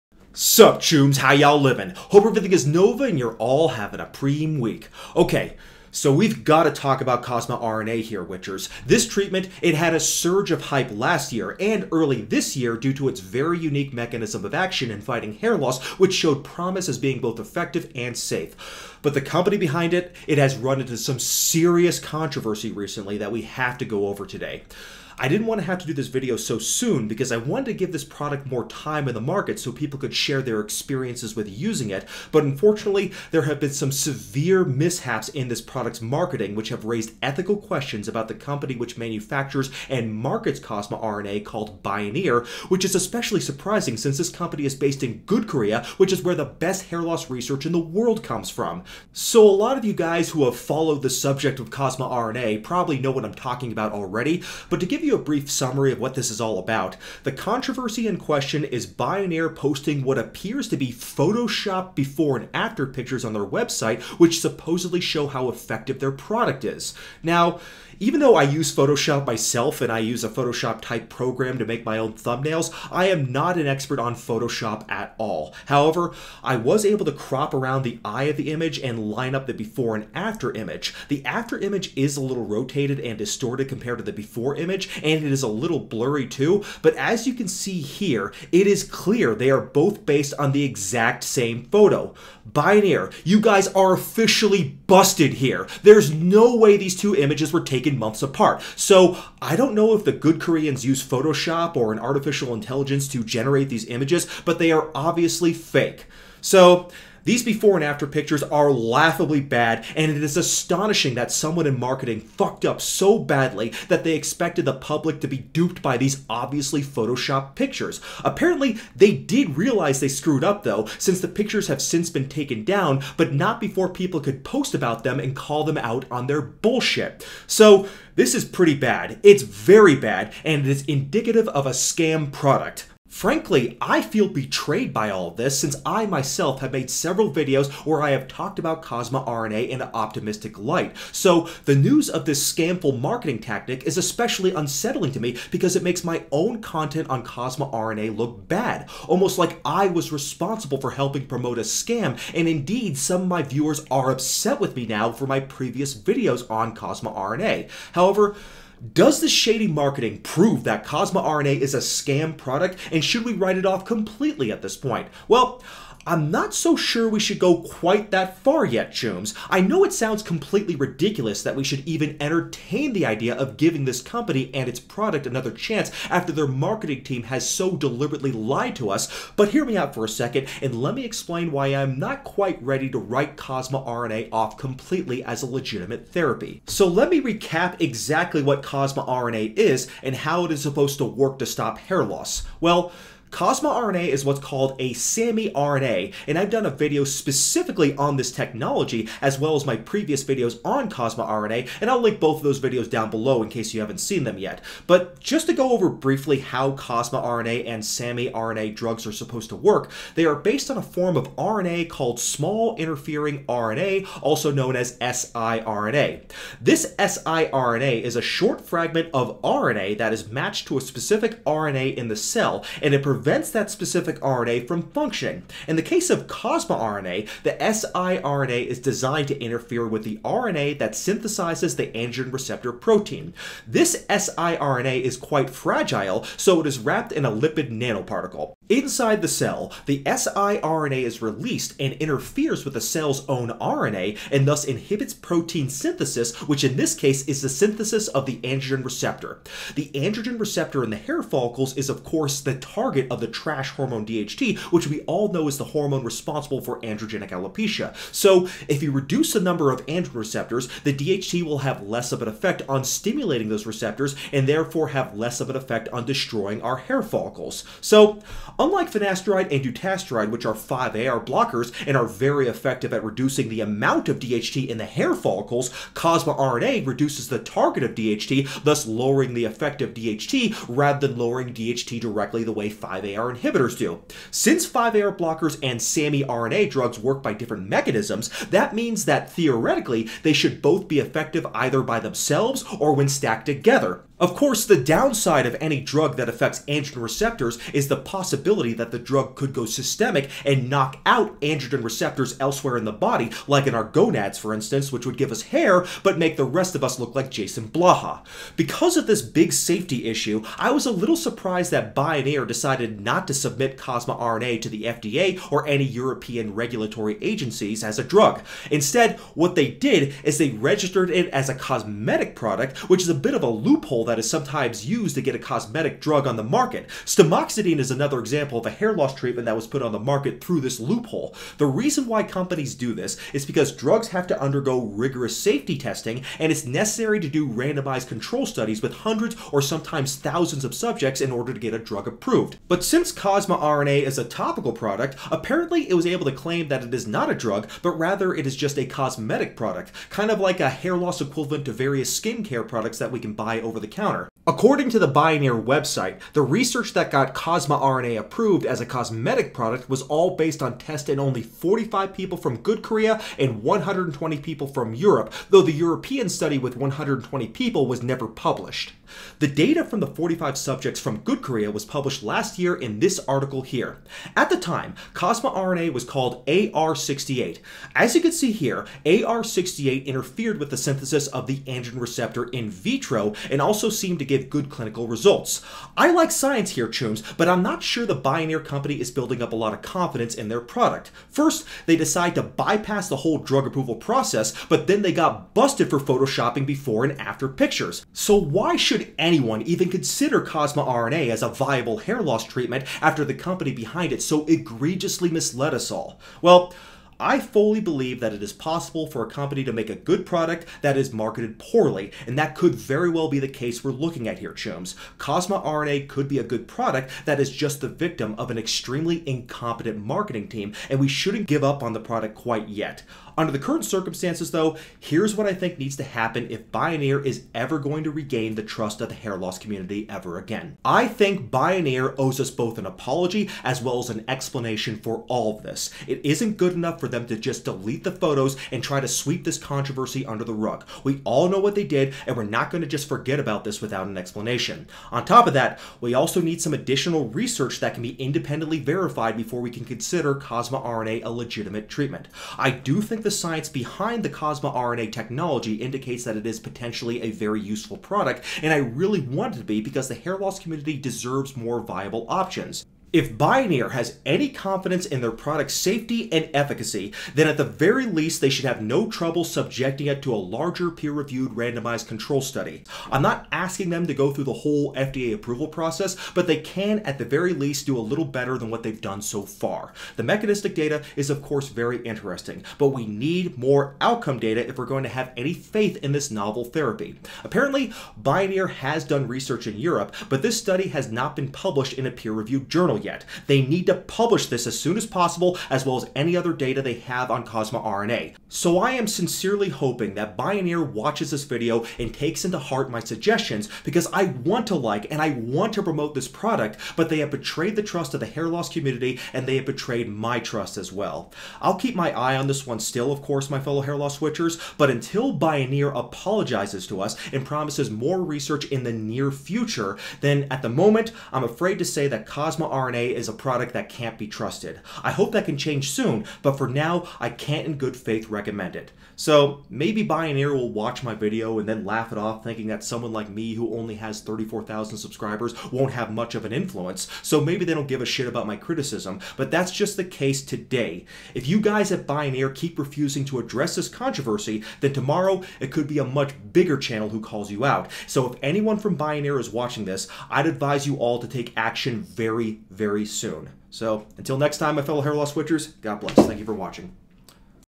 Is CosmeRNA a real hair loss treatment, or have we been scammed?
Sources:
1. Yun SI, Lee SK, Goh EA, et al. Weekly treatment with SAMiRNA targeting the androgen receptor ameliorates androgenetic alopecia. Sci Rep. 2022;12(1):1607. doi:10.1038/s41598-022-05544-w
2. Svoboda P. Key Mechanistic Principles and Considerations Concerning RNA Interference. Frontiers in Plant Science. 2020;11. Accessed February 15, 2022.